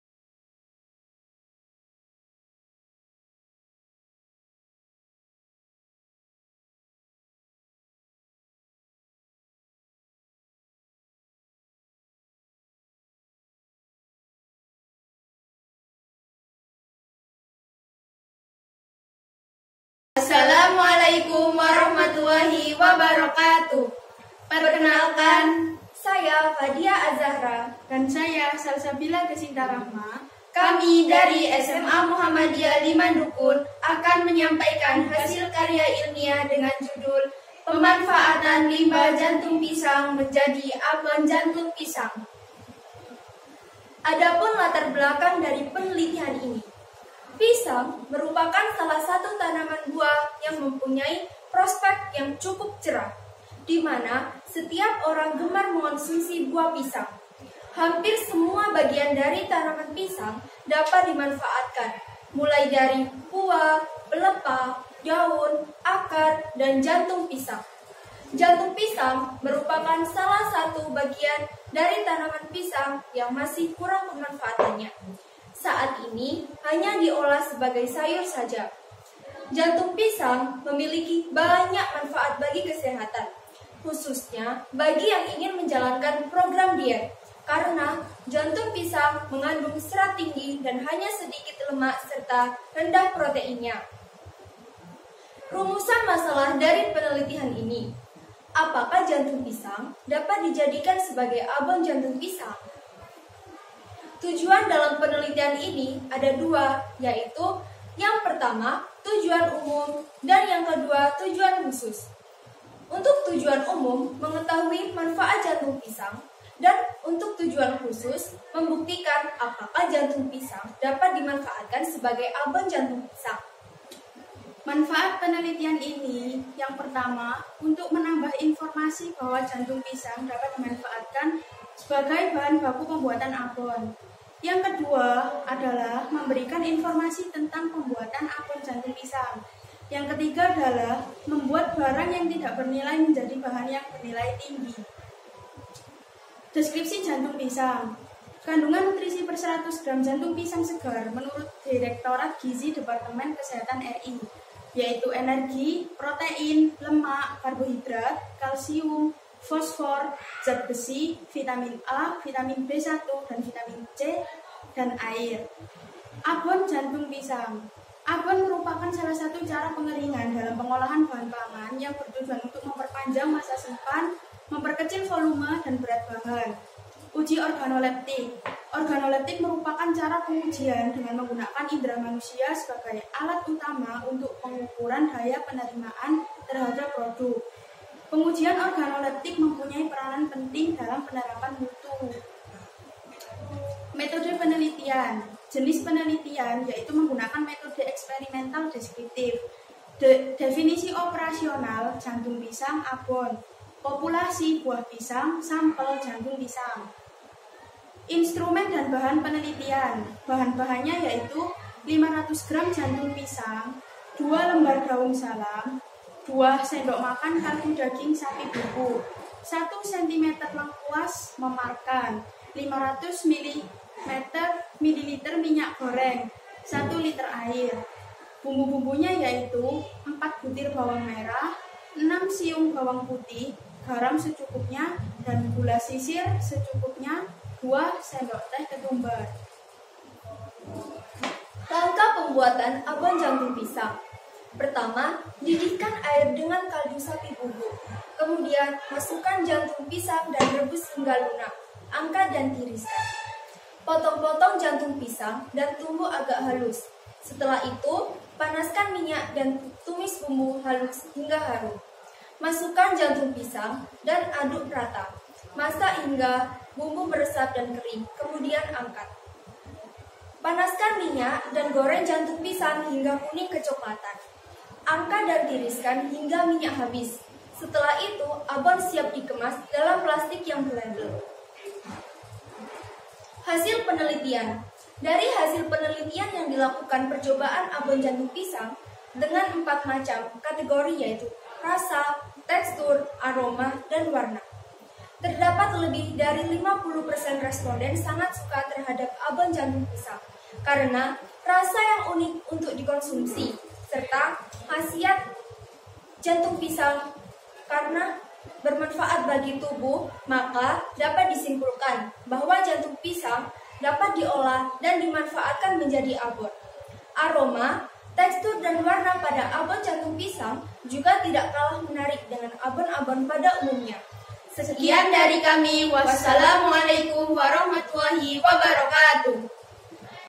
KTI FESTIVAL FAQIH USMAN #6 : 7. FADIA AZZAHRA & SALSABILA DESINTA RAHMA - SMA MUHAMMADIYAH 5 DUKUN
Festival Faqih Usman #6 Majelis Dikdasmen Pimpinan Daerah Muhammadiyah Gresik
KARYA TULIS ILMIAH : 7. FADIA AZZAHRA & SALSABILA DESINTA RAHMA  -  SMA MUHAMMADIYAH 5 DUKUN

Mohon selalu do’akan dan dukung program kerja kami agar semakin bermanfaat.
Utamakan sopan dan santun saat menulis di kolom komentar ya.
Terima kasih atas segala dukungannya.
Semoga bermanfaat :)